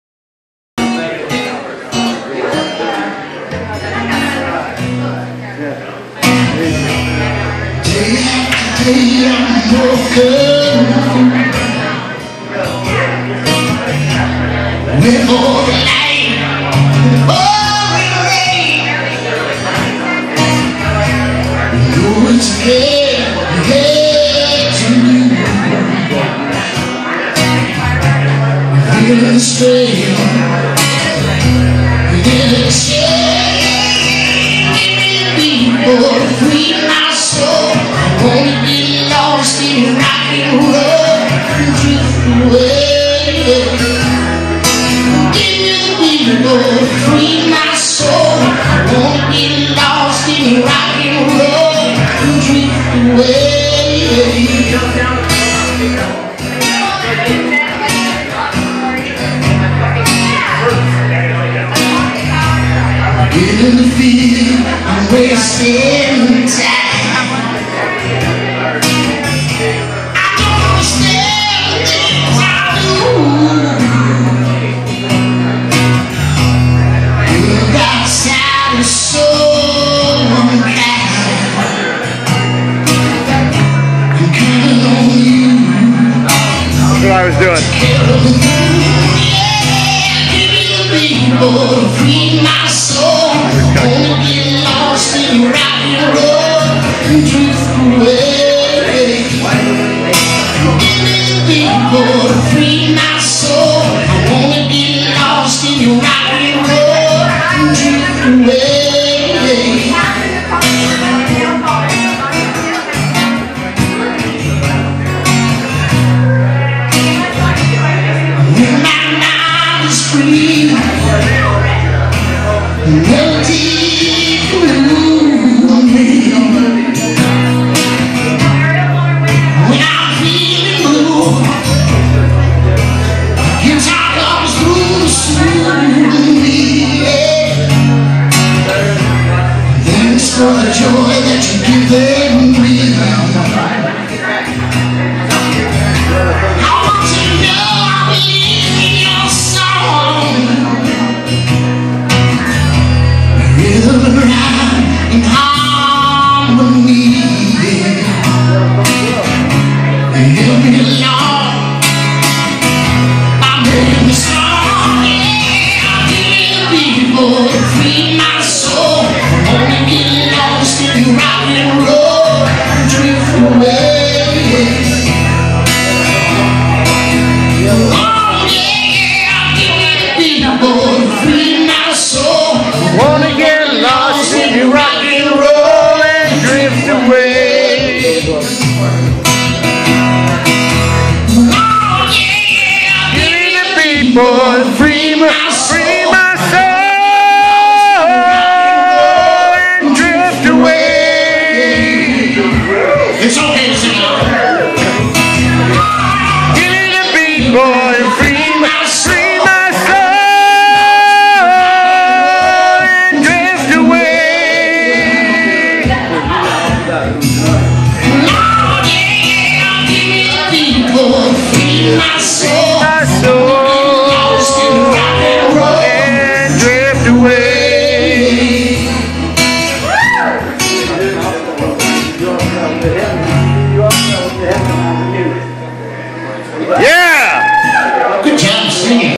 budmickle082713bar702
Bud Mickle performs at Bar 702 Open Mic Night. August 27 2013